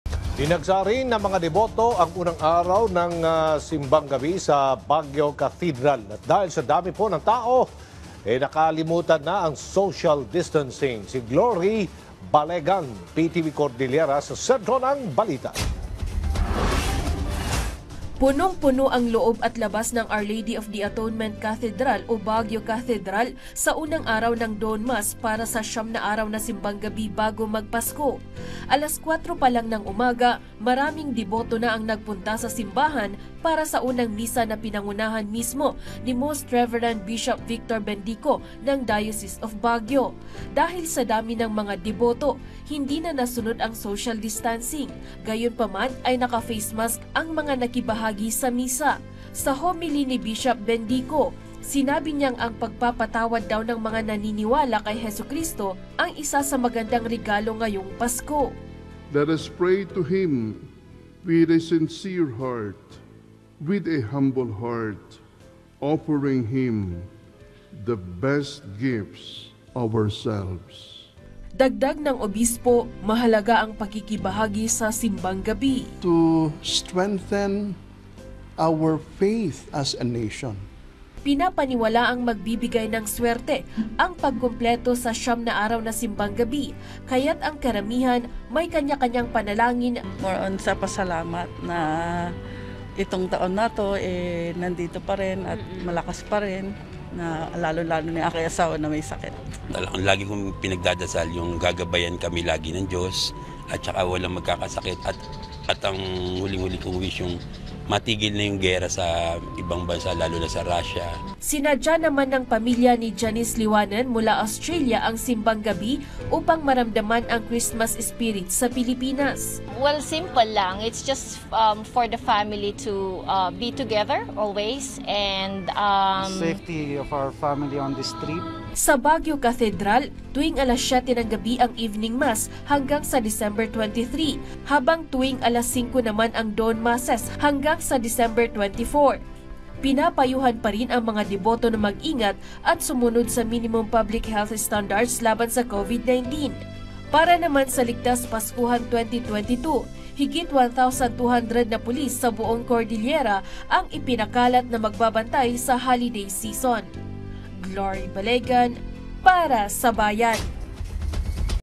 Baguio Cathedral, napuno ng mga deboto sa unang Simbang Gabi
Baguio Cathedral, napuno ng mga deboto sa unang Simbang Gabi; Mga deboto, pinapayuhan pa rin na mag-ingat at sumunod sa Minimum Public Health Standards